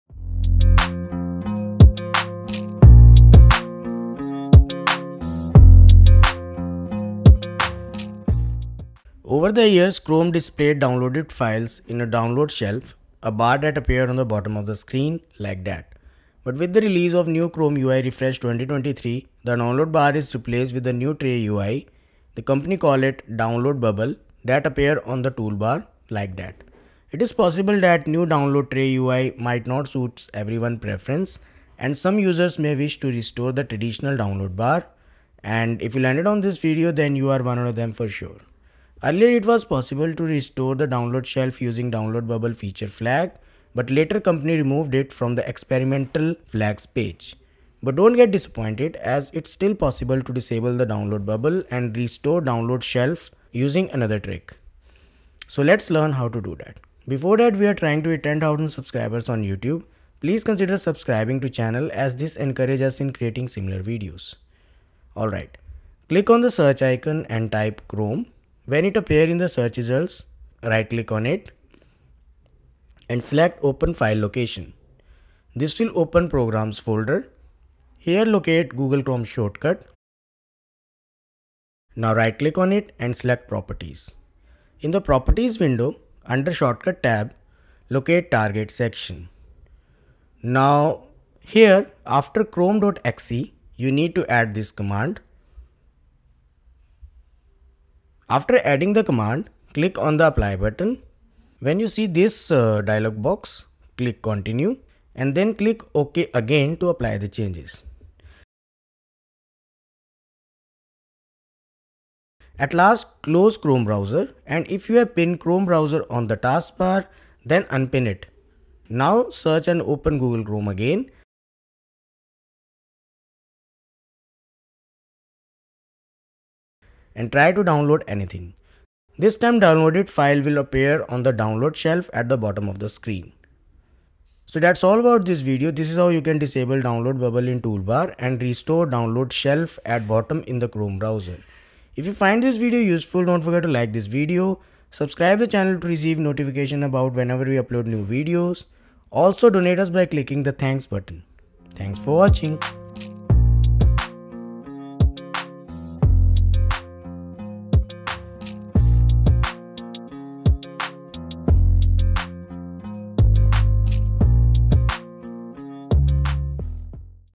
How to Disable Download Bubble Chrome & Restore Download Bar (Chrome)
If Chrome Download Bar Not Showing at Bottom, don't worry. In this video, I will show you how to Disable Chrome New Download Bubble and enable download bar or shelf in Chrome browser 2023 ui

WHATS INCLUDED IN THE VIDE0:

00:00 Introduction
00:48 Tutorial Starts from here
02:14 Outro

FOLLOW@GEEKERMAG
FOLLOW@GEEKERMAG
FOLLOW@GEEKERMAG

RECOMMENDED DOWNLOADS